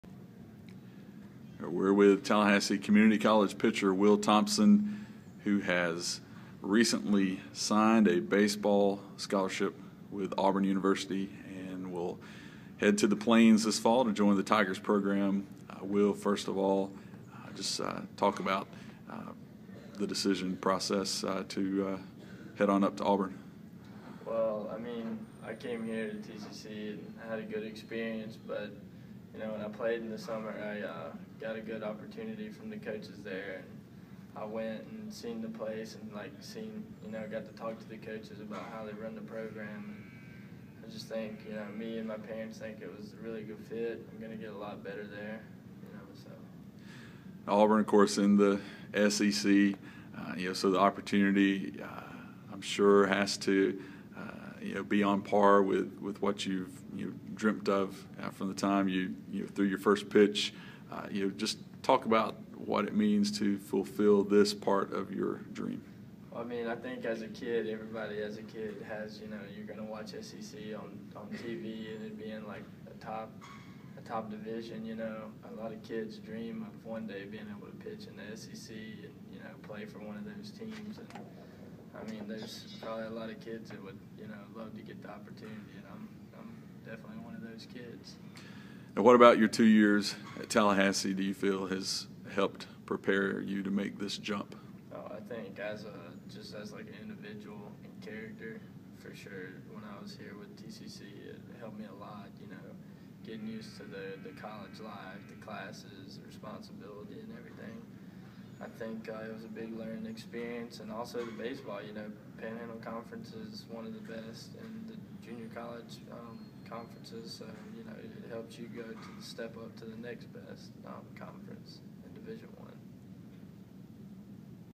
TCCBaseball's @will122793 signs
Lefty pitcher Will Thompson talks about his decision to sign a national letter of intent with the Auburn Tigers.